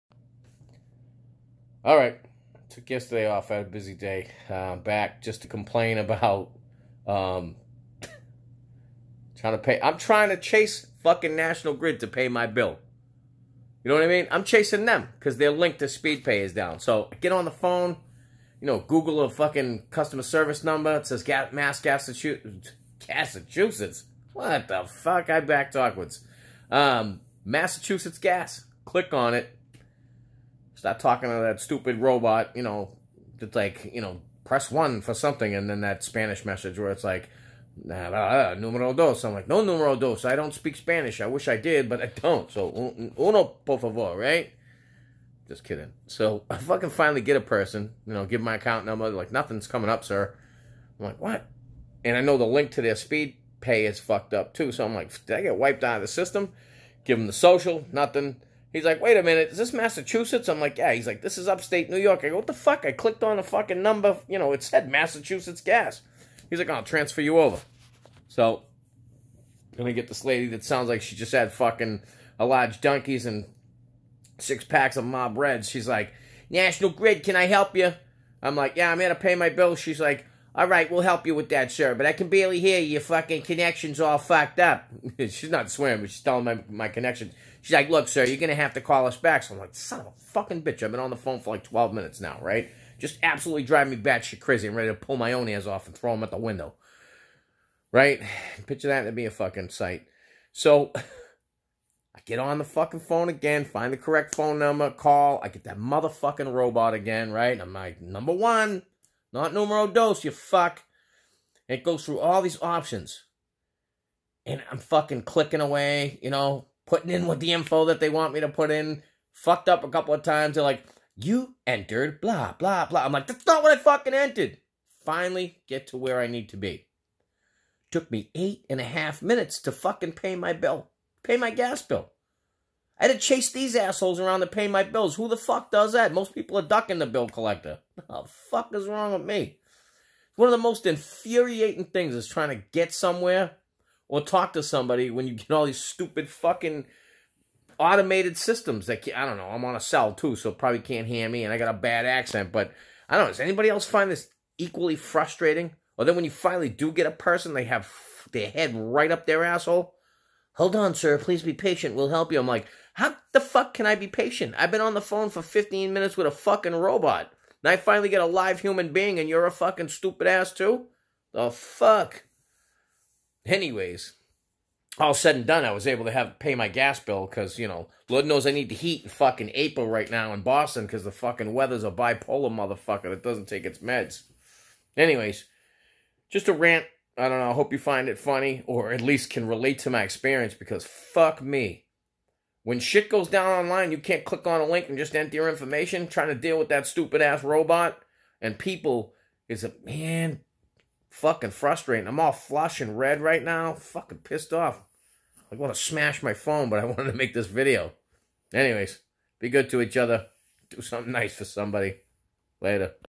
Calling nationalgrid raises my blood pressure!
Trying to pay my bill with a serious of robot and human interactions fml!